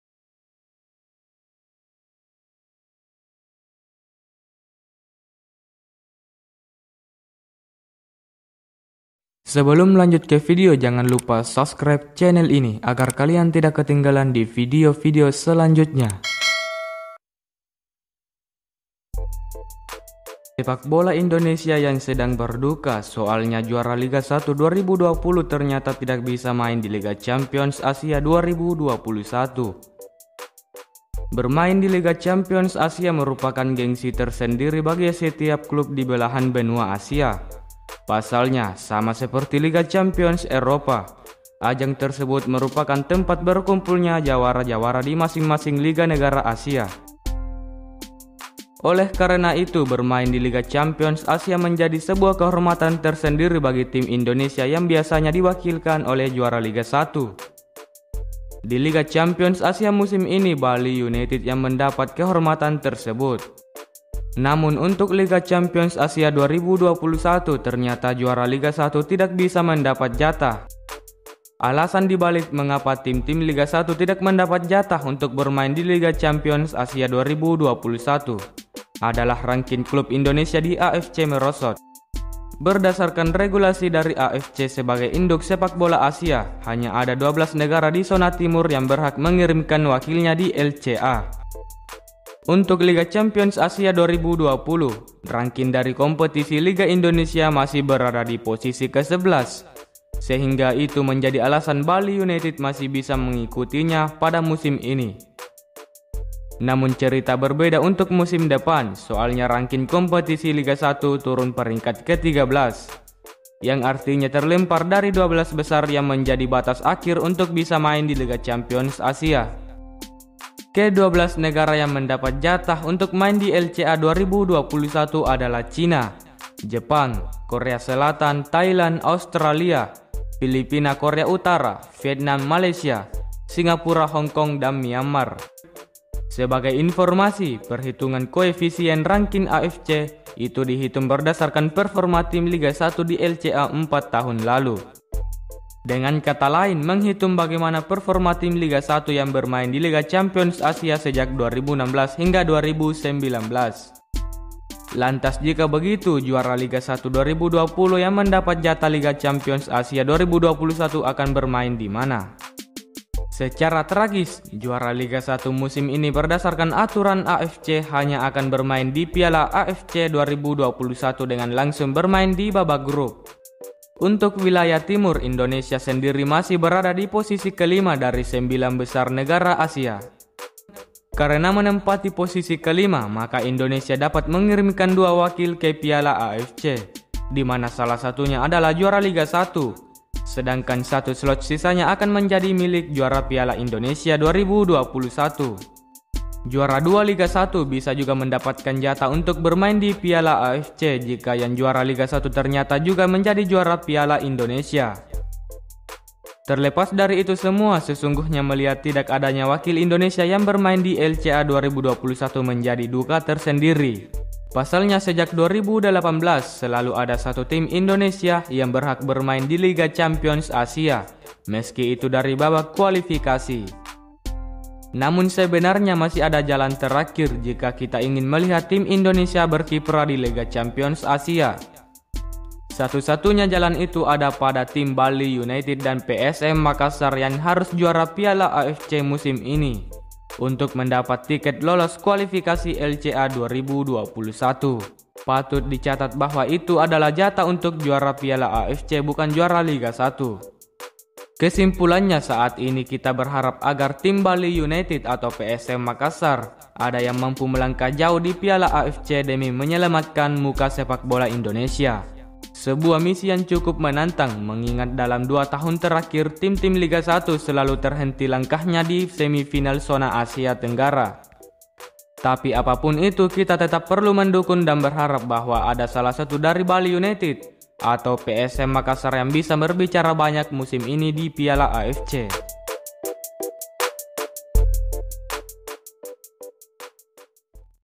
Juara Liga 1 Indonesia Tidak Mendapatkan Jatah di Liga Champions Asia 2021
Yukk berteman di instagram!!